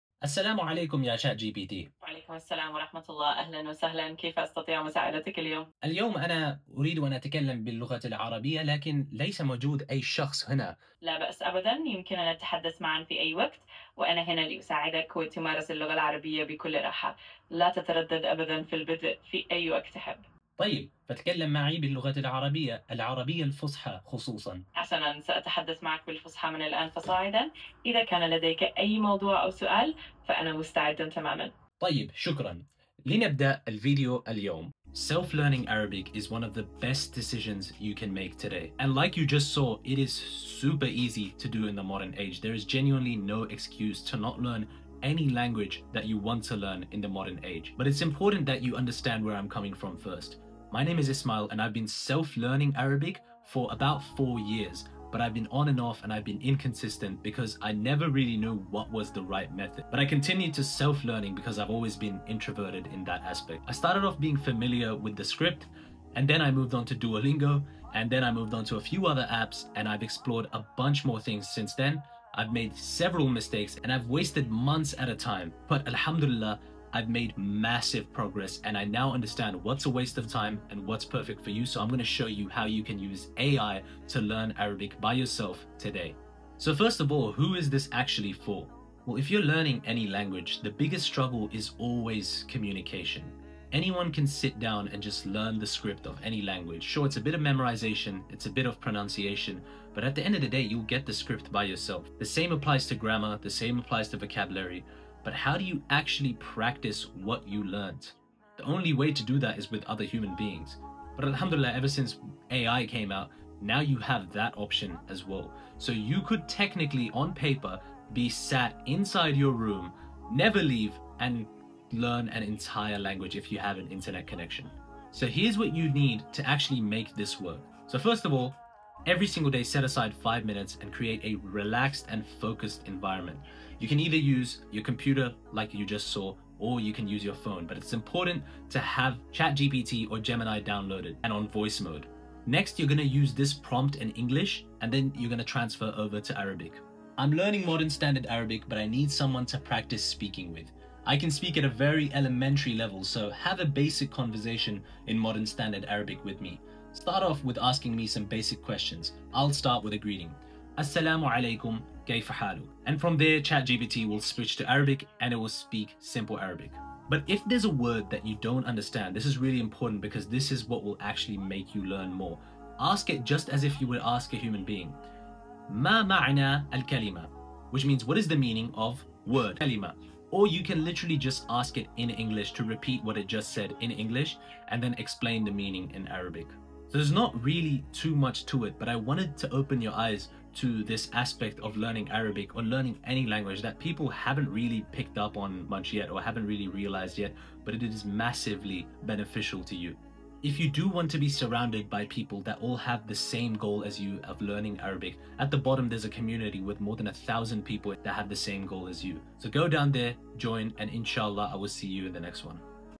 How I use AI to learn Spoken Arabic (ChatGPT, Gemini and more)
extremely simple guide, more people need to discover this ASAP.